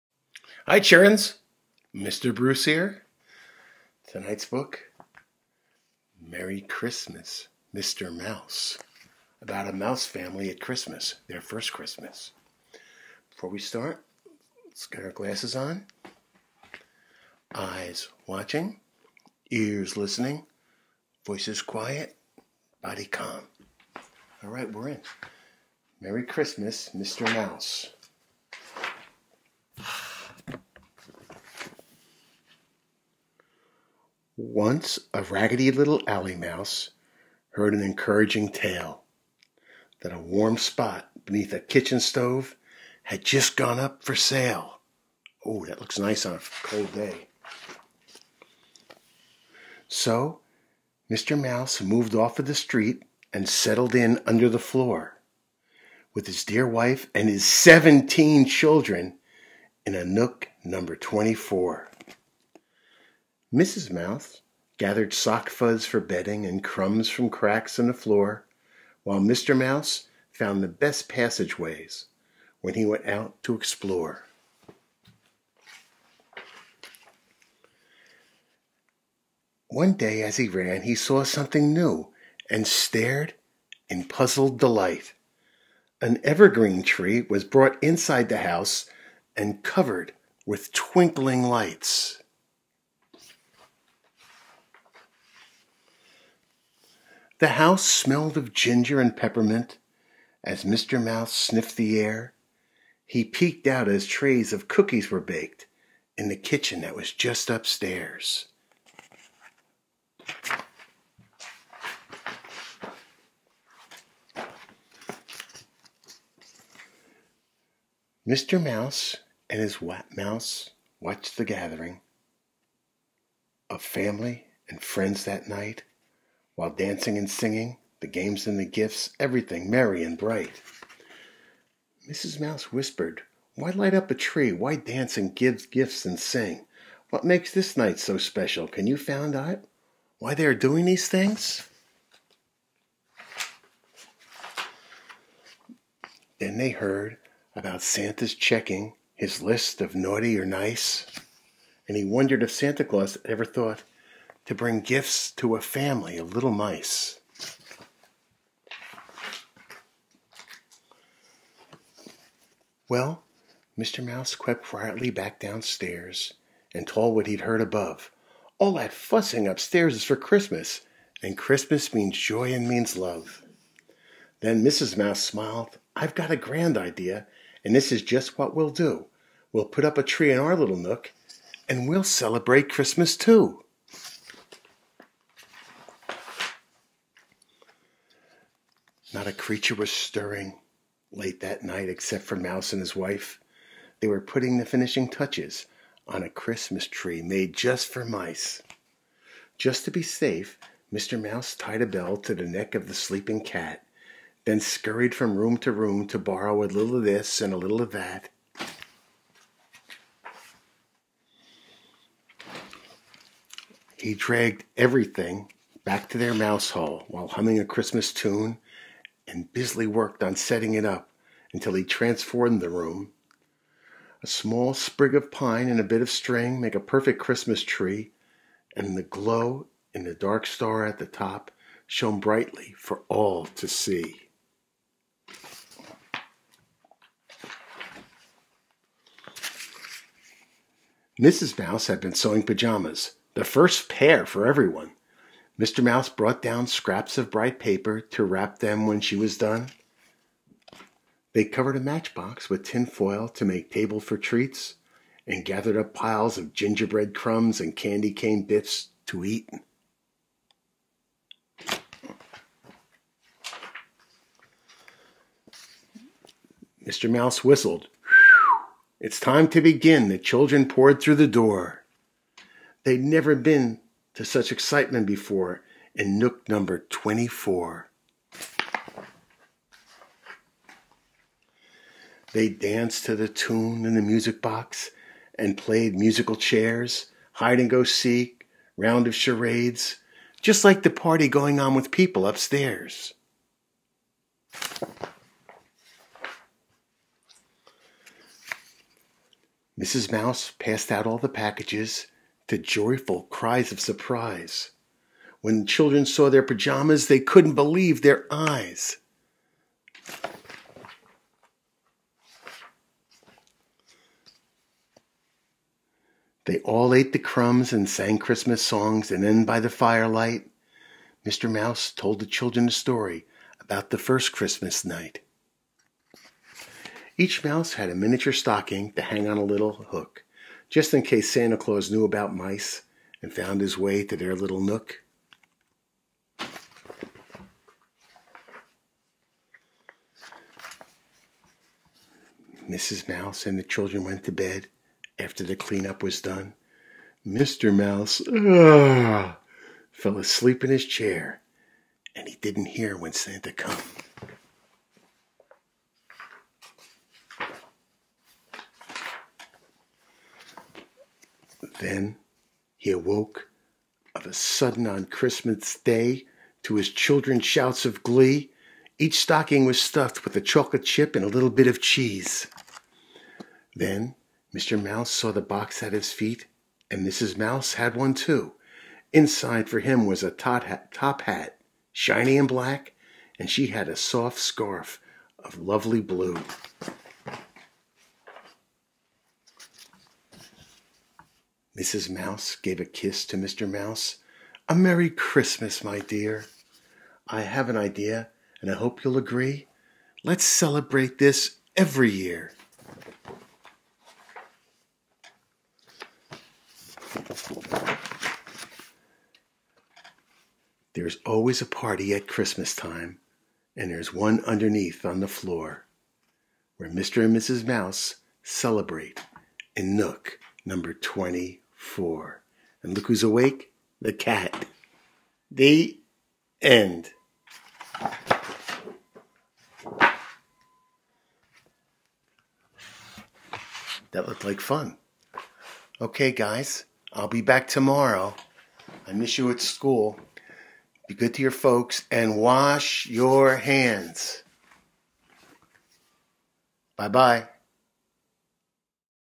Merry Christmas Mr. Mouse
Mr. Bruce reads "Merry Christmas Mr. Mouse" by Caralyn Buehner & Mark Buehner